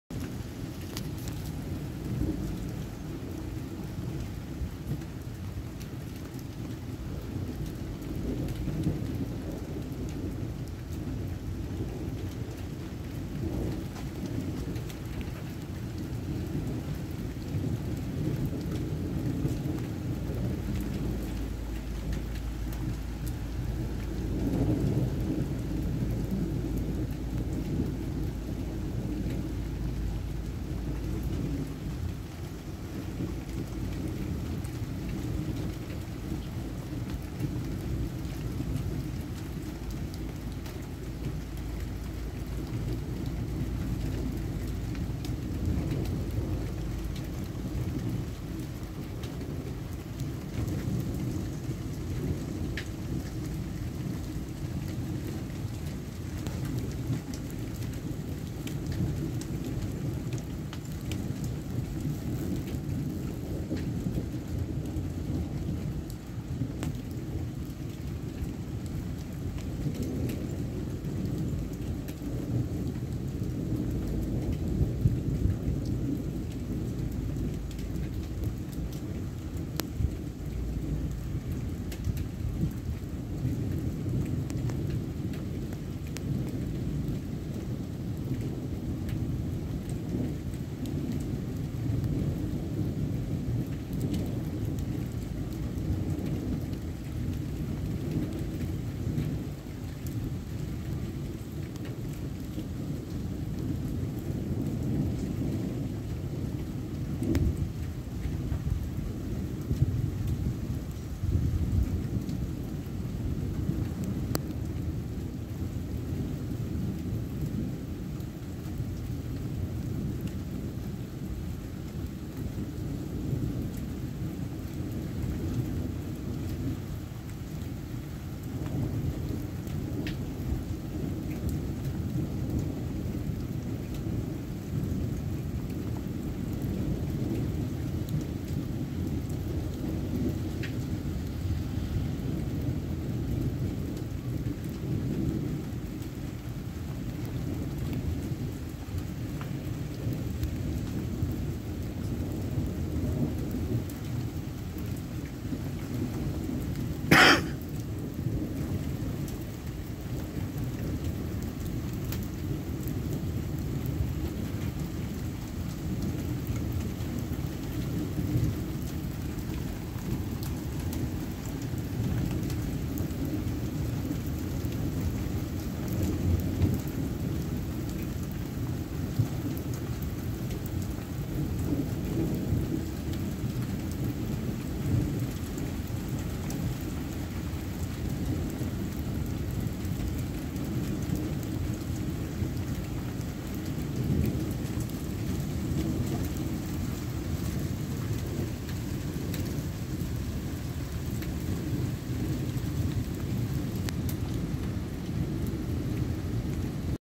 fin ally some rain July 24th 2021 Amery Wisconsin
it's been a hot dry summer hardly any rain this year. finally a good rain last night like 2:30 in the morning... video taken with my Samsung Galaxy a71.